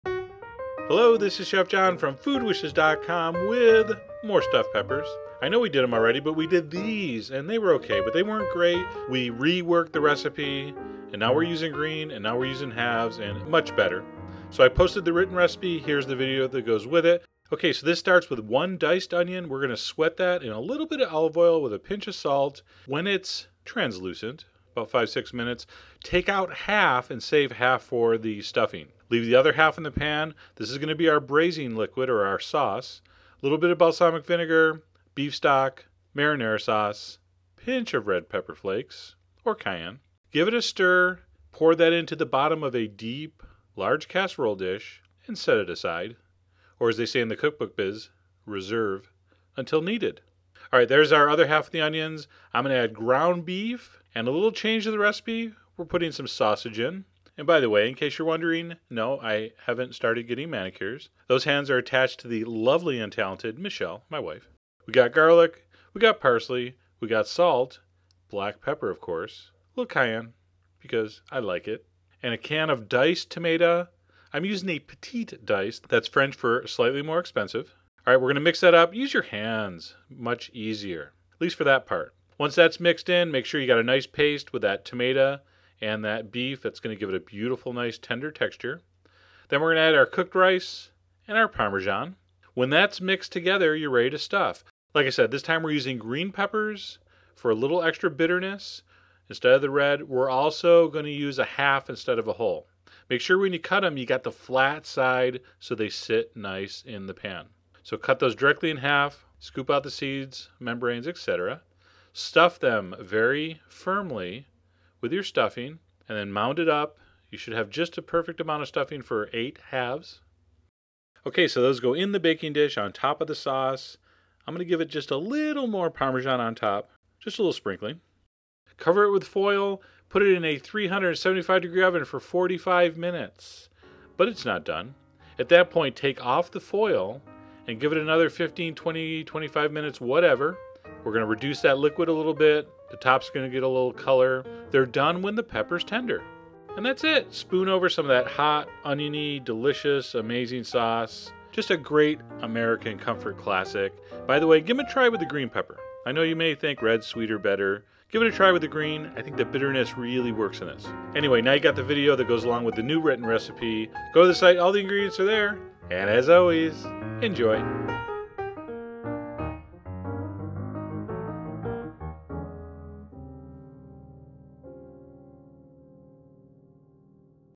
Food Wishes Recipes - Beef and Rice Stuffed Peppers Recipe - Stuffed Bell Peppers
Learn how to make a Beef and Rice Stuffed Peppers Recipe! to get more info, and watch over 350 free video recipes. Thanks and enjoy!